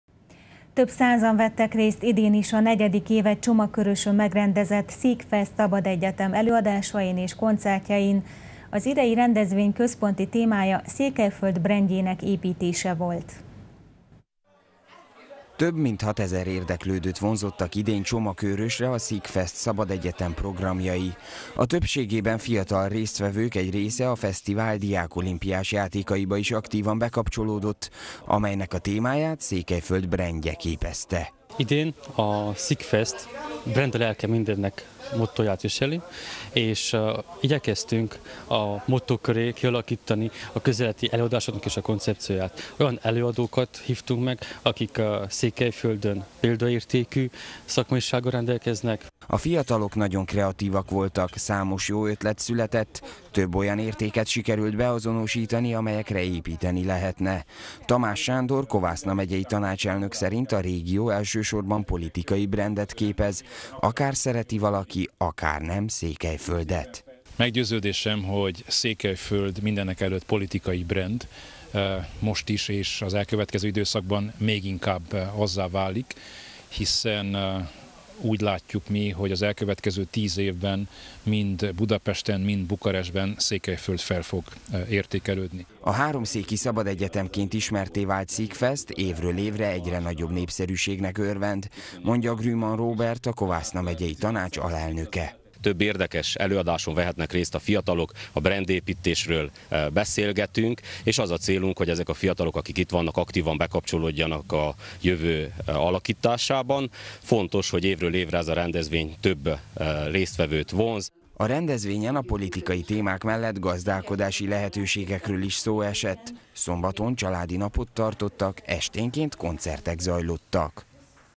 Székelyföld brandjét építették – Erdélyi Magyar Televízió
Több százan vettek részt idén is a negyedik éve Csomakőrösön megrendezett SIC feszt szabadegyetem előadásain, koncertjein. Az idei rendezvény központi témája Székelyföld brandjének építése volt.